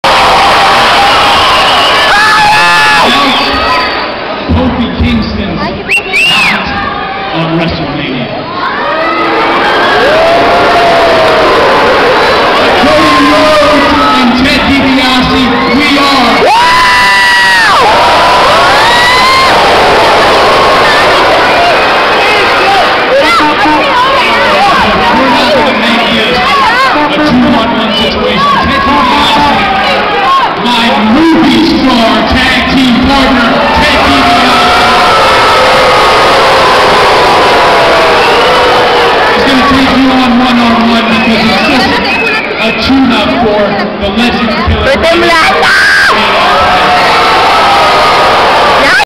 WWE Raw in El Paso, TX! 3/19/10
Oh Oh My My God God! Cody Rhodes and Ted Dibiase! but I went to see Cody Rhodes and he is sooooo Hot!